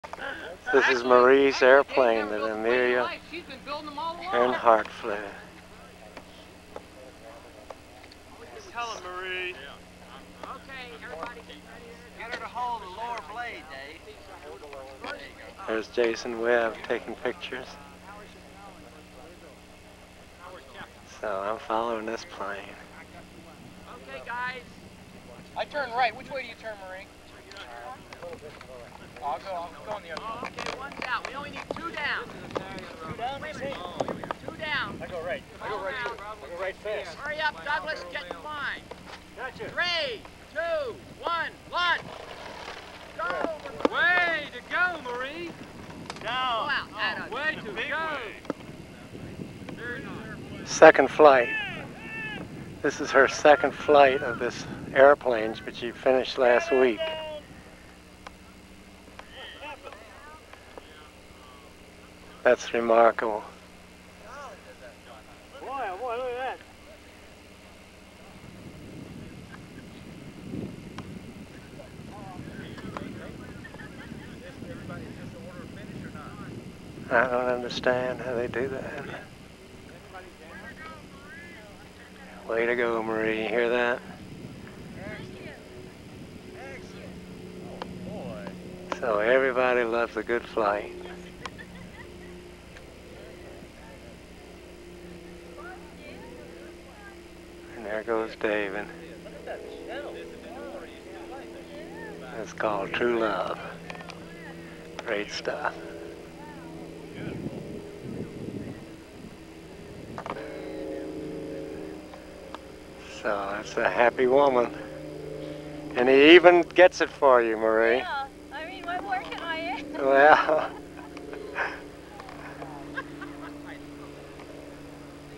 Marie & Dave Rees, Lockheed Vega, Kudzu 1992
Marie Rees flying her freshly built rubber powered Lockheed Vega under Dave's guidance. Classic F/F companionship during the 1992 Kudzu contest.Video by David Franks.Here they are in Geneseo 2004 with Marie's second Vega.